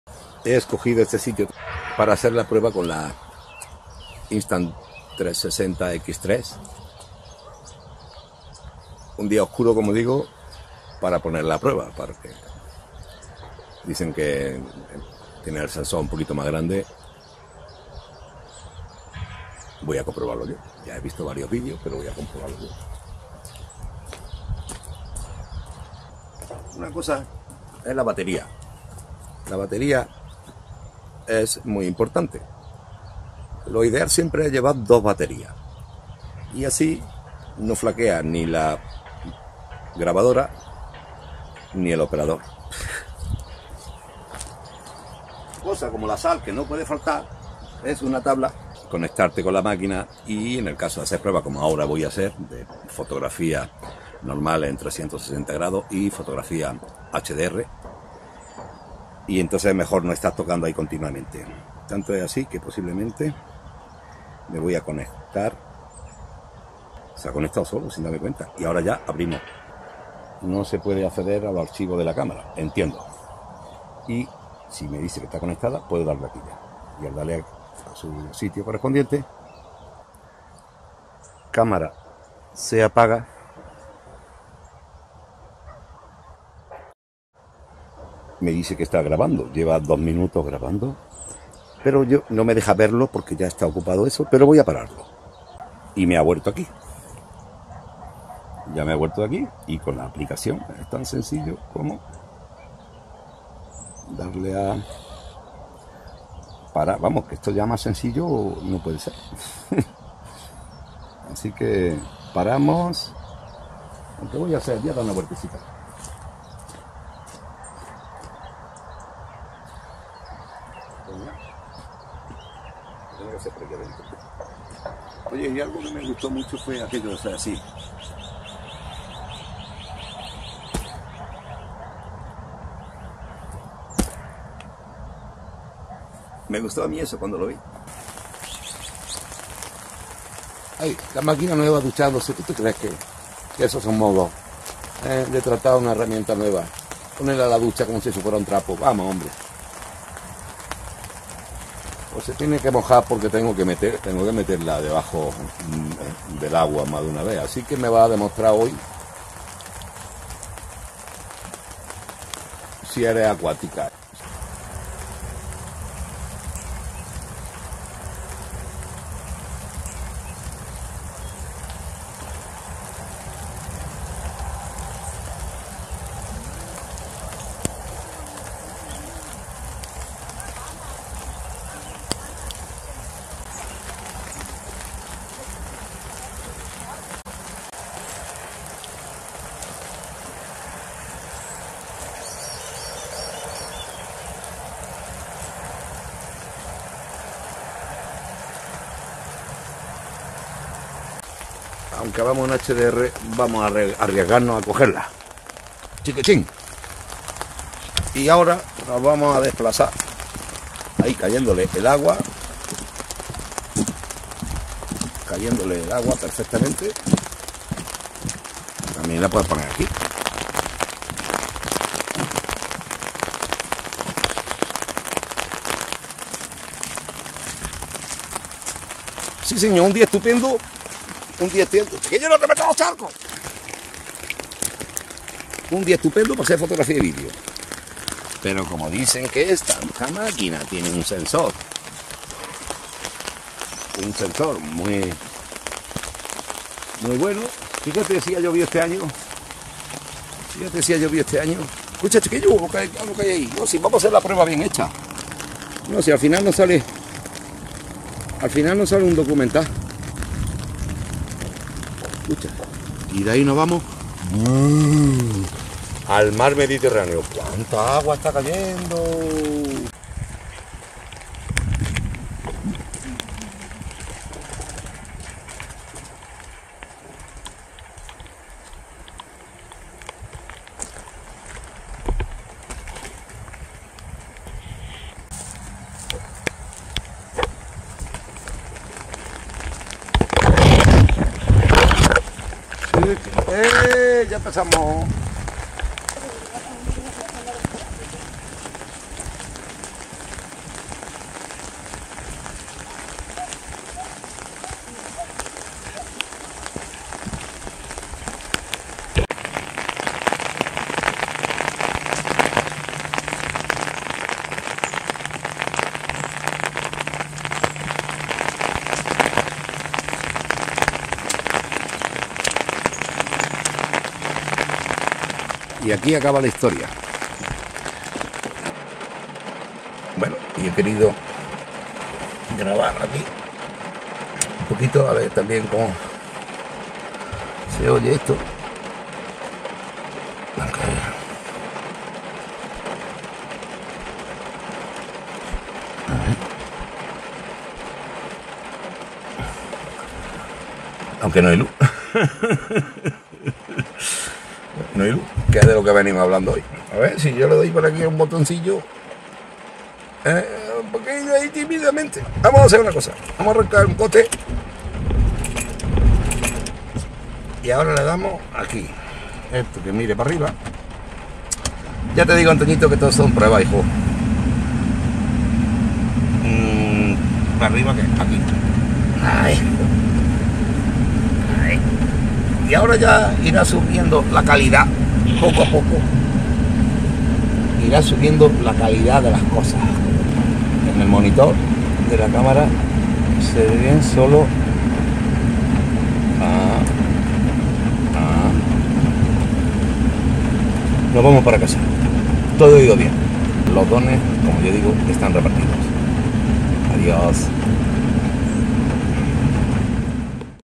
Cercado del Ciprés en Andújar Insta360 X3 con poca luz
Insta360 X3 con poca luz y lloviendo. Escojo el peor y a su vez el mejor día para estrenar con un vídeo de no mucho brillo la cámara Insta360 X3 que me he pompao

Escenario Cercado del Ciprés Andújar Jaén
Lince